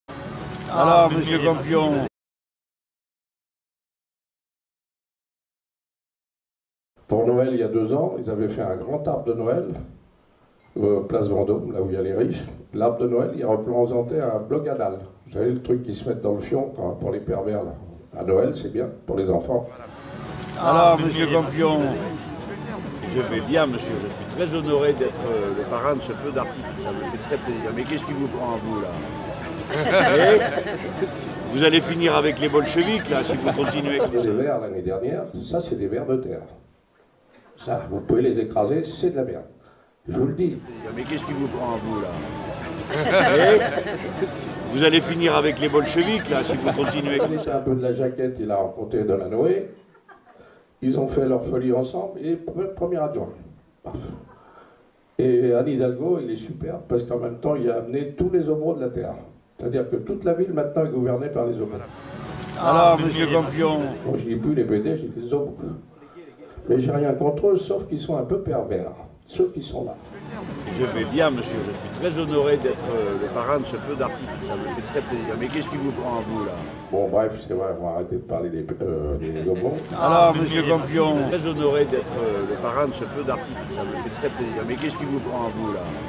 Mélenchon rend (presque) hommage à Marcel Campion pour ces propos homophobes !
Un grand visionnaire notre Jean-Luc, il avait déjà félicité Marcel Campion pour ses propos et ne pensait pas du tout à grappiller des voix !